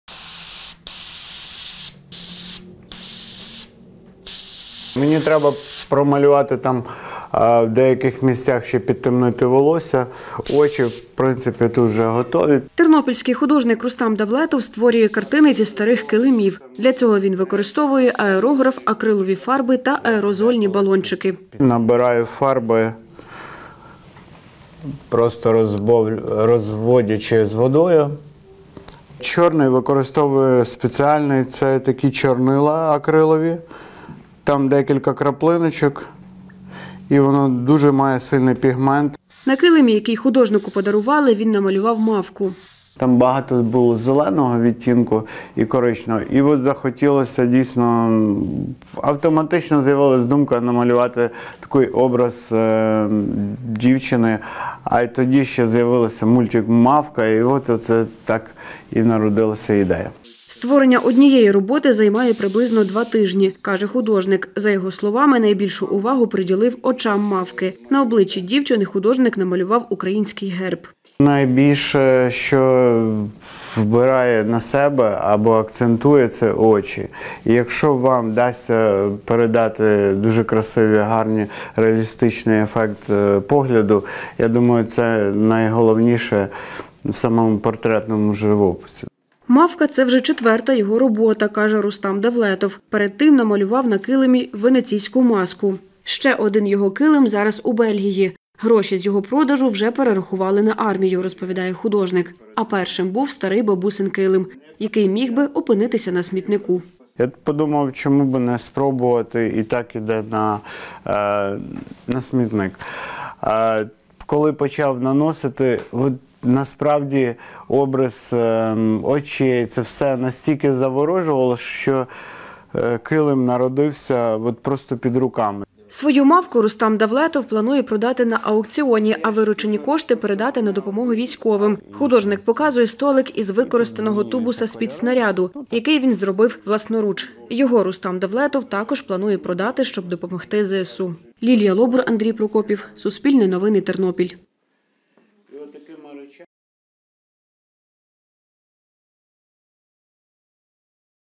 Тернопільський художник малює картини на килимах, щоб допомогти ЗСУ
Тернопільський художник Рустам Давлетов розфарбовує старі килими, щоб зібрати кошти для потреб українських військових. Одну з його робіт придбав українець з Бельгії, а виручені 10 тисяч гривень перерахував для ЗСУ. Щоб перетворити килим в арт-об’єкт, художник використовує власну технологію. Про це – у сюжеті.